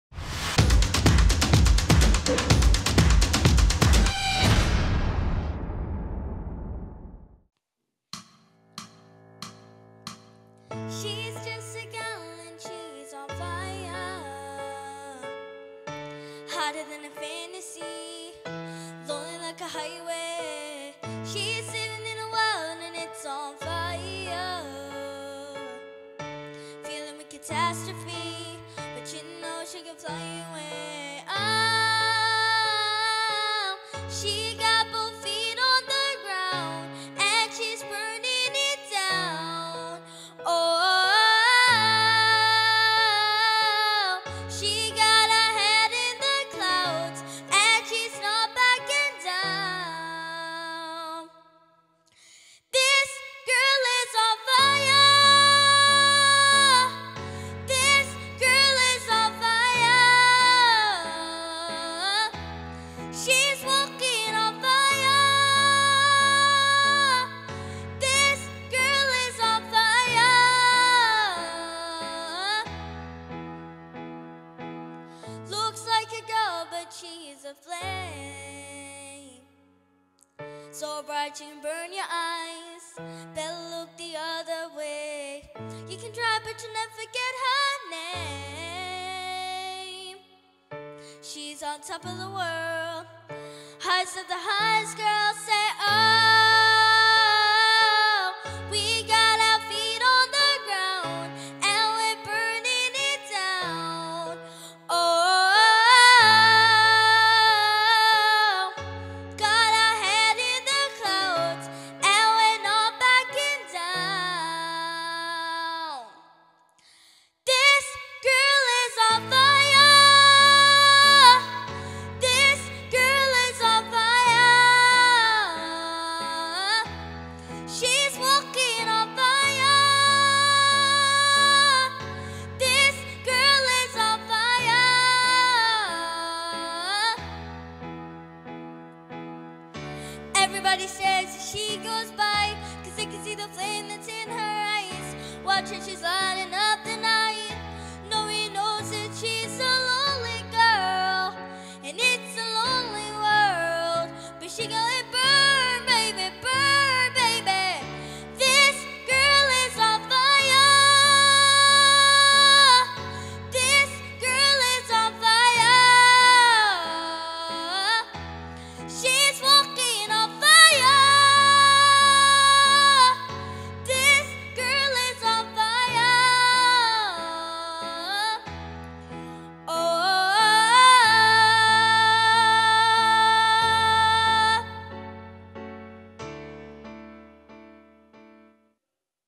Emelia | SHAPE Your Talent 2022 - Auditions | 8 - 13 Age Category
Hi, my name is Emelia and I am 9 years old.  I love performing arts and I am currently in a Theatre School.   My favourite thing to do is singing, I have been singing for as long as I could talk.  I was super excited when I saw this talent competition and knew it was exactly what I wanted to do.
Shape Your Talent is great for kids in the area to do what they enjoy doing.  In the future, I would love to enter more singing competitions and my dream would be to sing in a West End performance.
It would be great if people were to vote for me as it would boost my confidence and help me realise I can achieve my dreams If I work hard and win through.